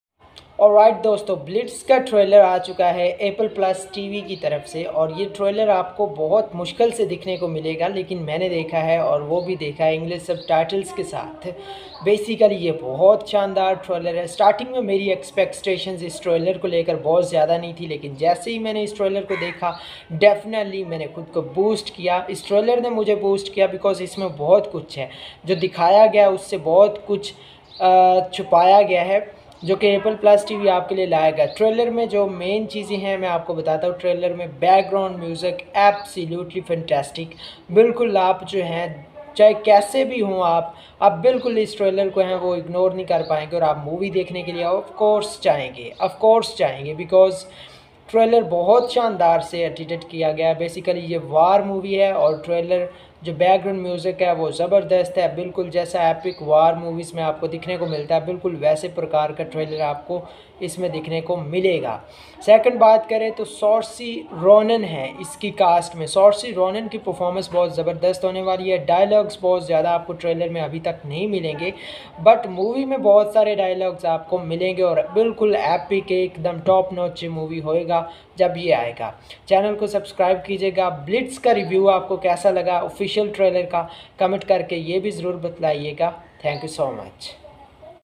Blitz | Saoirse Ronan Blitz (Movie 2024) Official Trailer Review 1080p | Movie Review in Hindi
Description:-

Blitz Official Trailer has Outed by Apple+ T.V. and I have watched and Reviewed this Trailer in Hindi. Saoirse Ronan Performance is great to watch in this movie. By Genre Movie is War and Drama Movie.